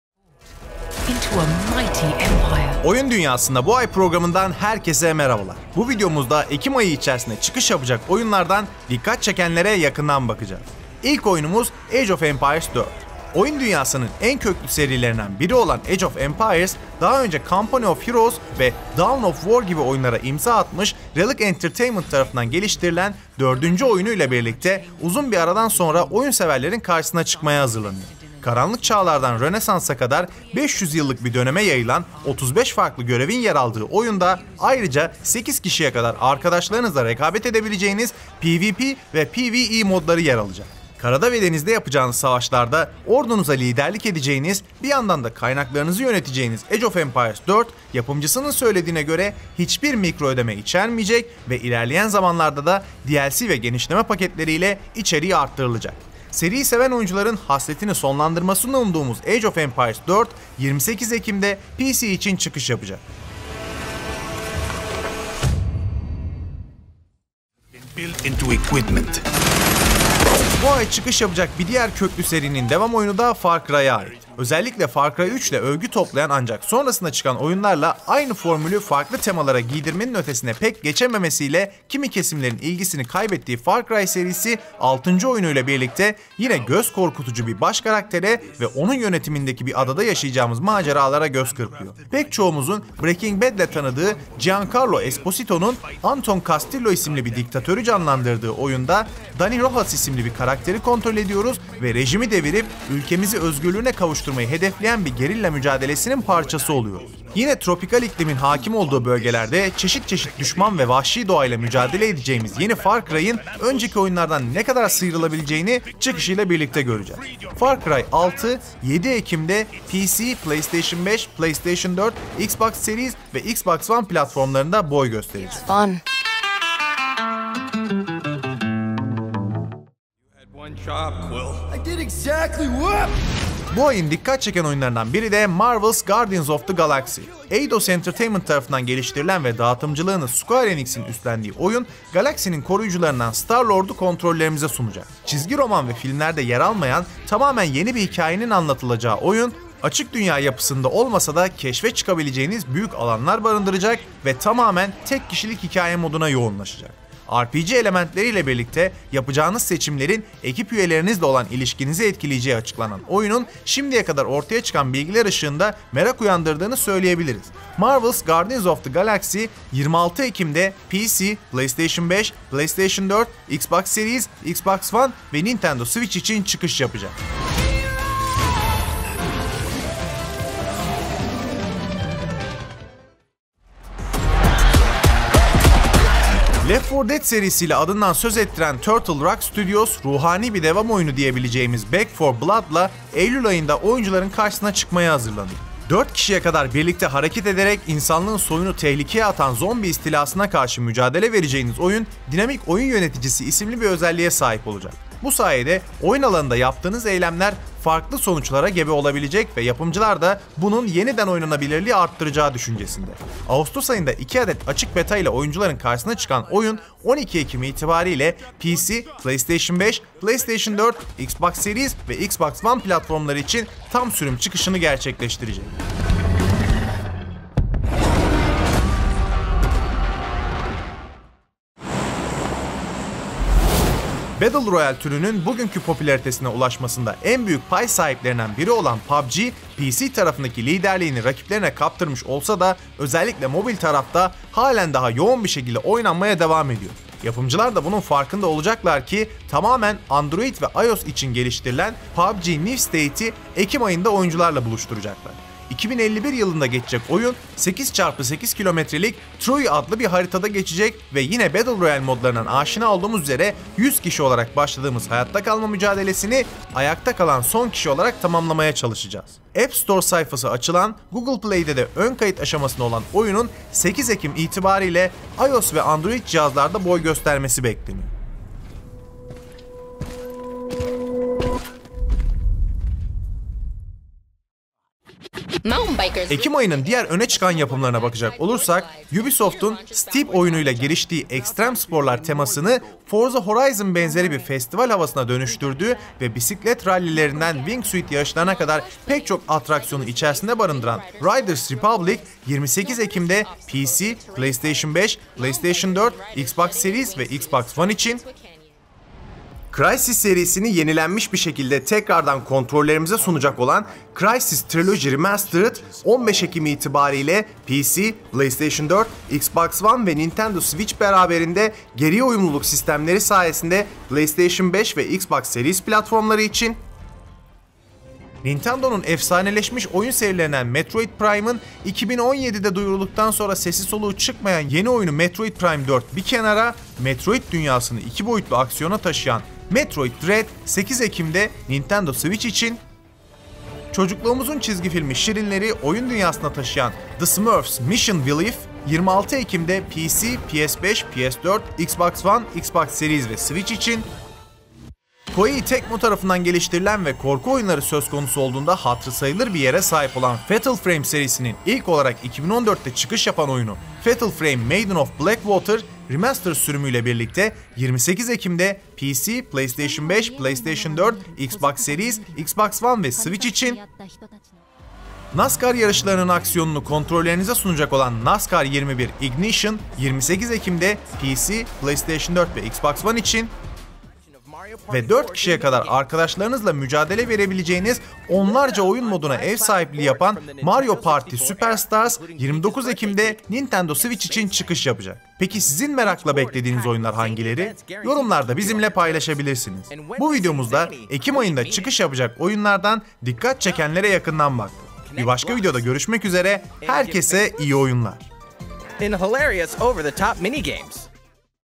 Oyun Dünyasında Bu Ay - Ekim 2021
Oyun dünyasında bu ay programından herkese merhabalar. Bu videomuzda Ekim ayı içerisinde çıkış yapacak olan oyunlardan bazı dikkat çeken yapımlara yakından bakıyoruz.

Türkiye'nin en büyük Teknoloji Portalı Donanimhaber.com'un Youtube kanalına hoşgeldiniz. Güncel teknoloji haberleri için sayfamızı ziyaret edebilirsiniz.

Bizi aşağıdaki hesaplarımızdan takip edebilirsiniz;